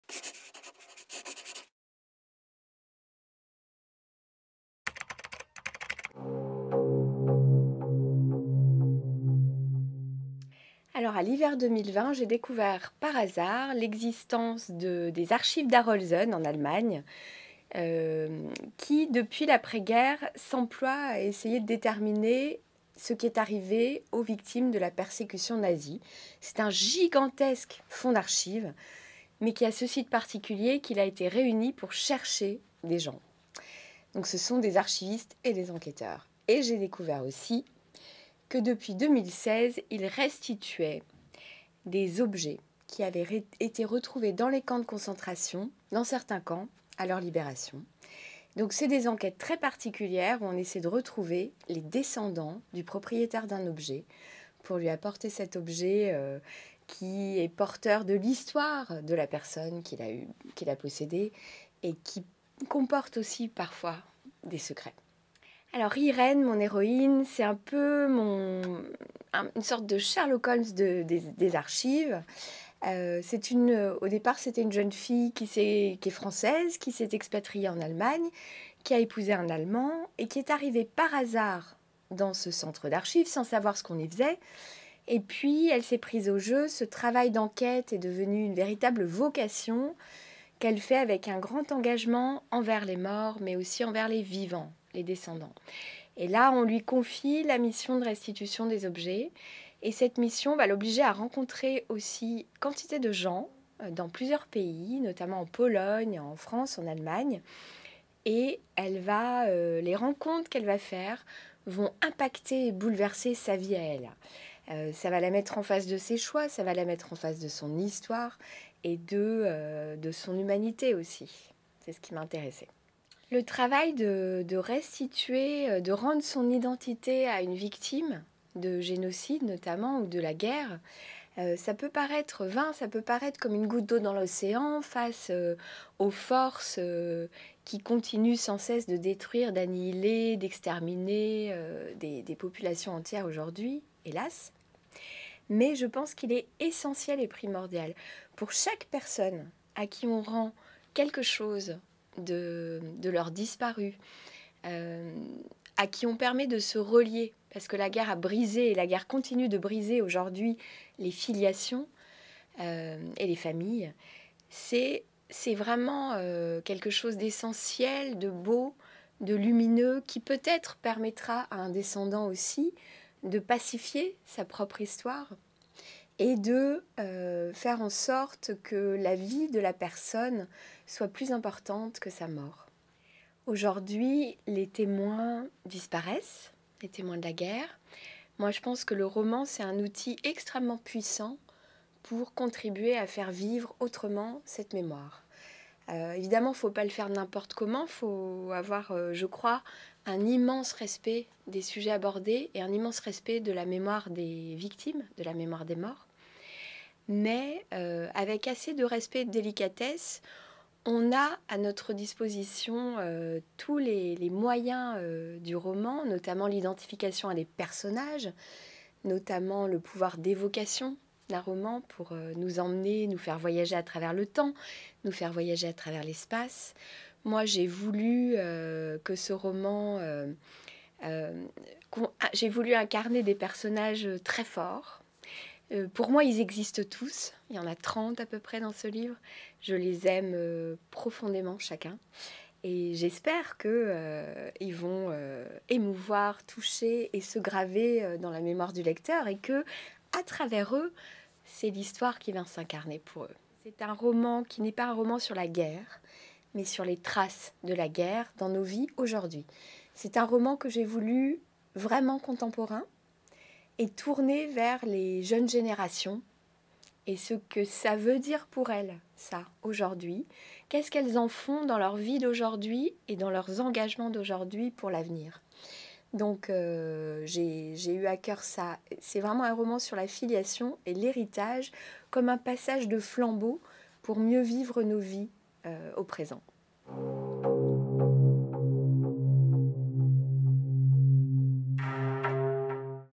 Gaëlle Nohant - Le bureau d'éclaircissement des destins
Gaëlle Nohant vous présente son ouvrage "Le bureau d'éclaircissement des destins" aux éditions Grasset. Rentrée littéraire janvier 2023.